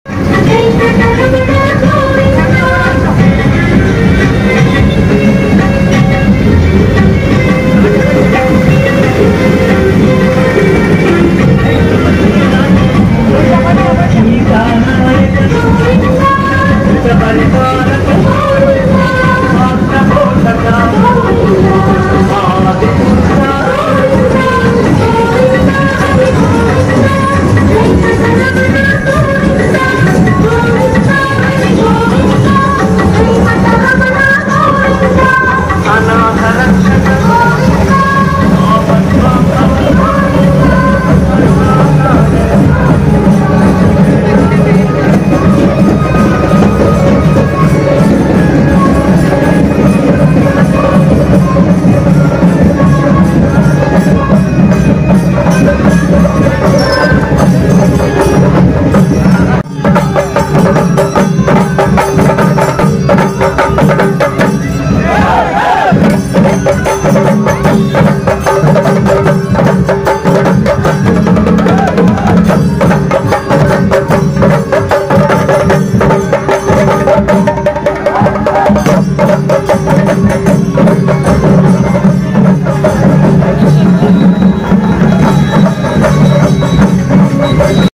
लक्षवेधी मिरवणूक|परळीत तिरुपती बालाजीचे प्रत्यक्ष दर्शन अन् आवतरले शिवकथा प्रवक्ते पं.प्रदीप मिश्रा
Ganeshotsav: गणेशविसर्जन लक्षवेधी मिरवणूक | परळीत तिरुपती बालाजीचे प्रत्यक्ष दर्शन अन् आवतरले शिवकथा प्रवक्ते पं.प्रदीप मिश्रा